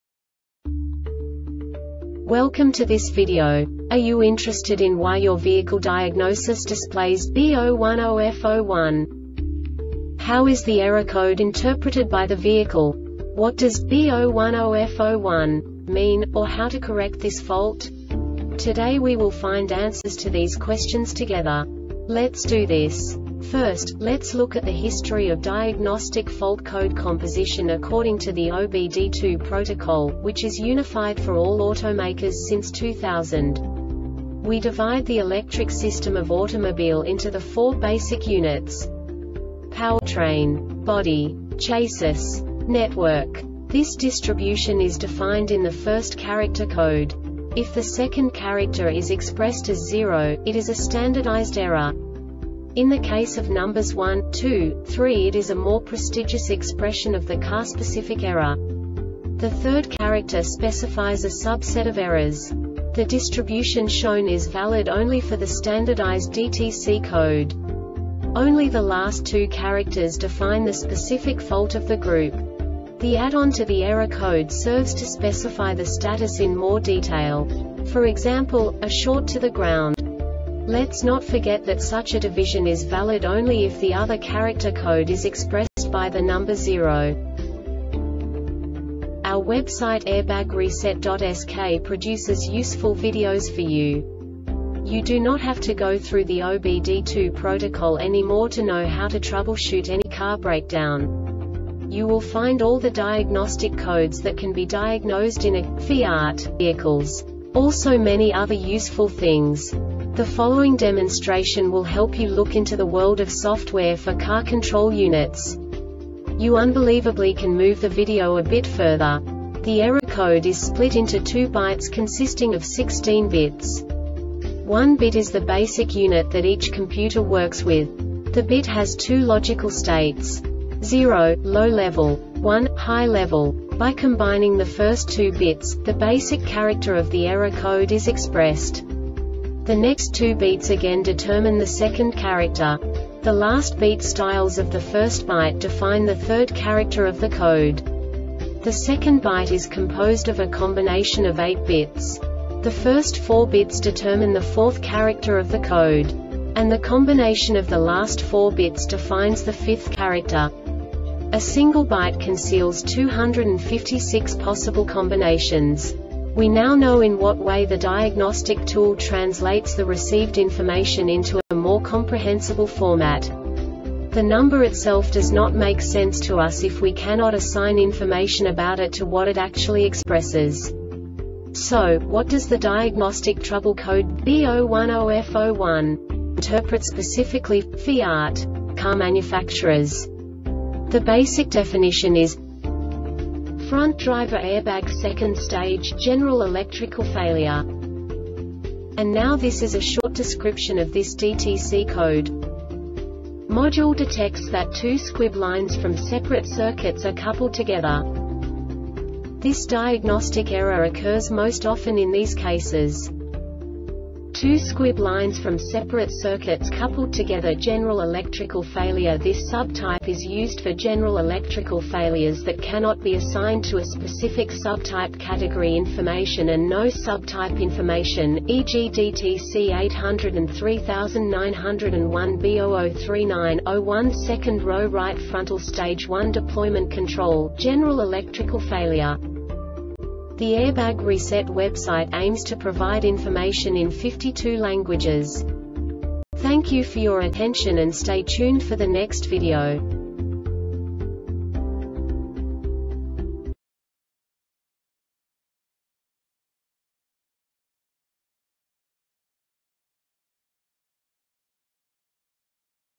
DTC Fiat B010F-01 Short Explanation
Contents:
0:21 Basic DTC analysis according to OBD2 protocol standard.
1:48 Insight into programming
2:58 A diagnostically understandable description of the Diagnostic Trouble Code
3:05 Basic error definition

Make:
FIAT
Code:
B010F-01
Definition:
Front Driver Airbag 2Nd Stage - General Electrical Failure
Description:
Module detects that two squib lines from separate circuits are coupled together.
Cause:
2 Squib Lines From Separate Circuits Coupled Together
Failure Type: